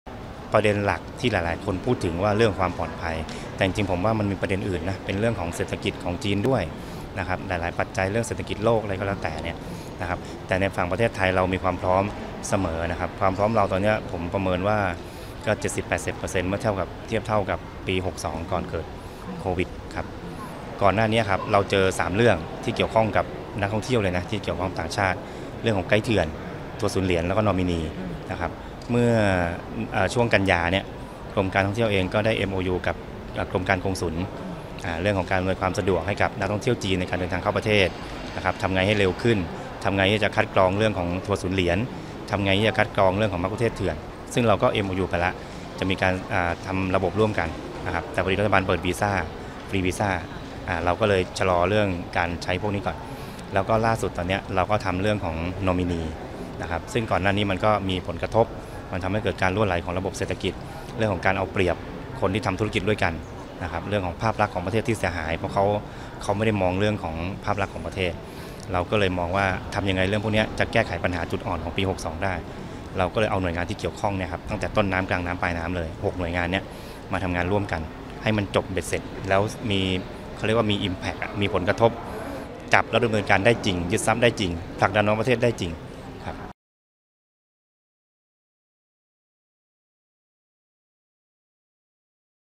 เร่งแก้ปัญหานอมินี ฟื้นความเชื่อมั่นการท่องเที่ยว | Thai PBS News
ปัญหานอมินี ทัวร์ศูนย์เหรียญและไกด์เถื่อน ส่งผลกระทบต่อผู้ประกอบการท่องเที่ยวและภาพลักษณ์ของประเทศ จำเป็นต้องเร่งแก้ไขร่วมกับหน่วยงานที่เกี่ยวข้องอย่างจริงจัง เพื่อสร้างความเชื่อมั่นหวังดึงนักท่องเที่ยว 35 ล้านคน ในปีหน้า ขณะที่ปีนี้คาดว่าจะอยู่ที่ 28 ล้านคน